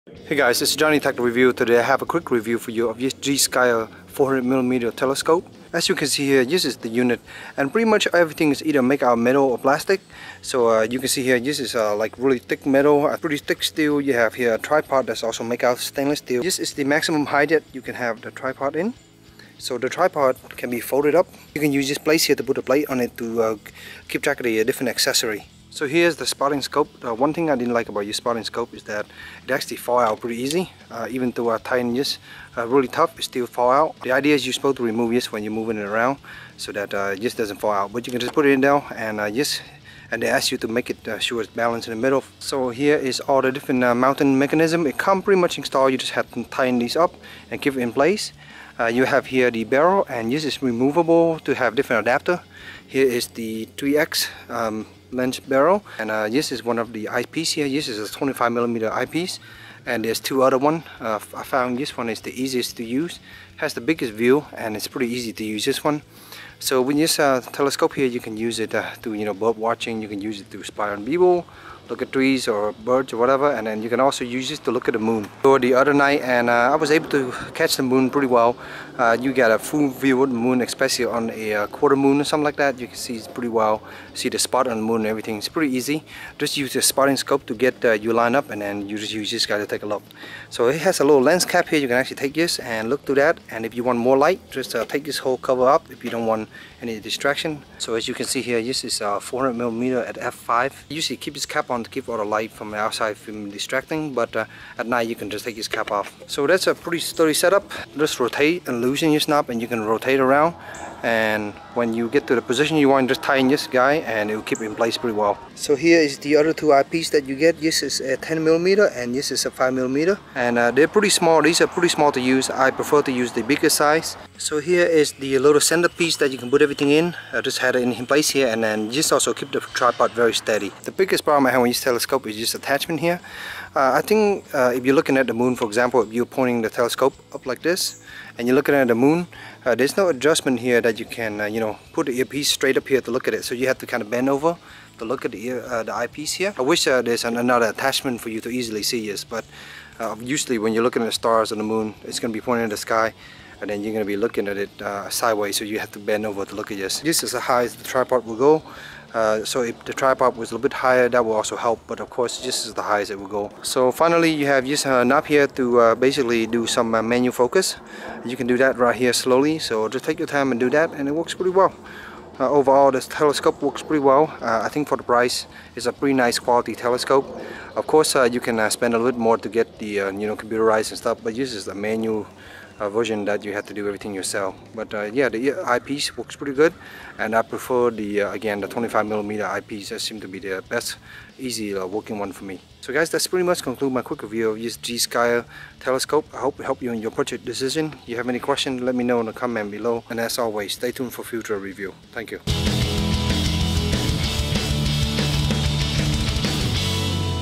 Gskyer 400mm Astronomical Refracting Telescope REVIEW
- Metal and plastic build, good quality but isn't super heavy
- 400mm is decent to see the moon, checking out stars is more tricky
- Come with 3 eye pieces, my favorite is 25mm size as it's easier for me to see in it
- Tripod get about 4.5' high, it's annoying to have to bend over to look into eye piece, I wish they gave a better eye barrow attachment
Overall it works pretty well and quickly fold up into smaller size for transport. If they give better eye piece for 90 degree viewing it would be great. A hard carrying case would be nice also as this is small and portable to carry